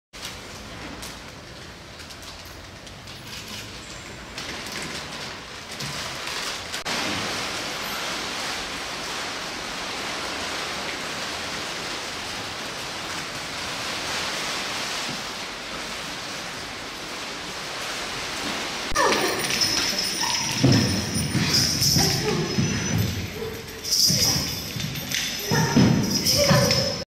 Trailer Edenica proximas fechas
Edénica

Anyelén Demichelis /Alejandro Lonac/ Gabriel Lugo /Mónica Menacho

Iluminación: Gonzalo Monzón

Dirección: Diana Rogovsky

Registro de Cirila Luz Ferron en el marco de la sexta edición de Danzafuera Festival